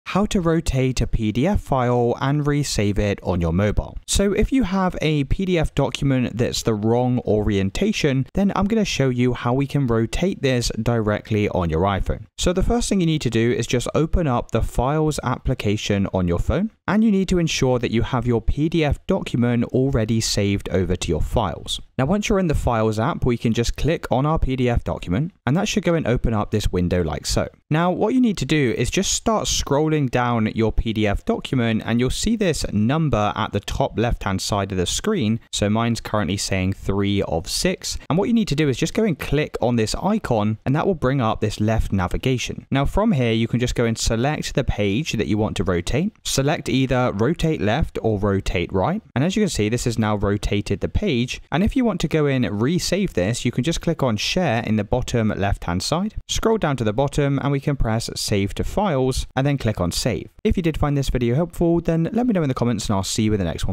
How To Rotate PDF File & Save in Mobile (iPhone)
In this guide I will show you how to rotate a PDF file directly from your iPhone. If your PDF document is the wrong orientation then we can change it from Landscape to Portrait or Portrait to Landscape directly from our phone and resave it back to our files.